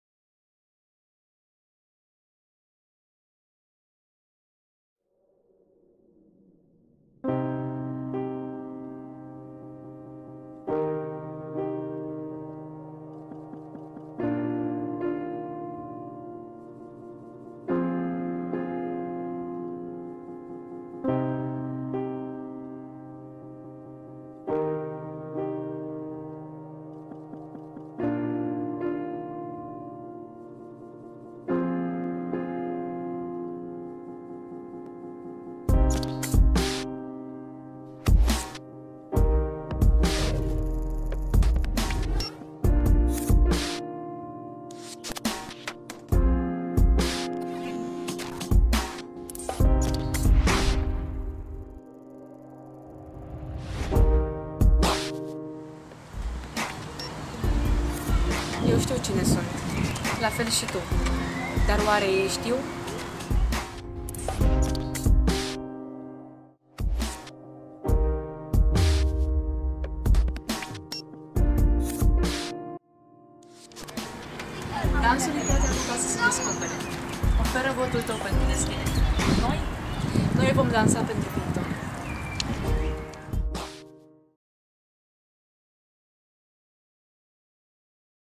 SIA Romania 2014 Finalist: Dansăm pentru VIITOR!
Proiectul își propune să vină în întâmpinarea nevoilor de auto-cunoaștere, de înțelegere a propriei lumi, precum și de dezvoltare a abilităților de viață pentru tinerii aflați în centrele de plasament. Datorită tehnicilor de dans contemporan, îmbinate cu exerciții de dezvoltare personală, tinerii instituționalizați vor putea să-și exploreze potențialul, să găsească soluții alternative, pentru problemele întâmpinate, și să identifice  puncte de interes cu ceilalți participanți. Atelierele vor facilita baza pentru cunoaștere interpersonală și crearea de parteneriate între tineri, pentru ca aceștia să se integreze mai usor în societate, după părăsirea centrului.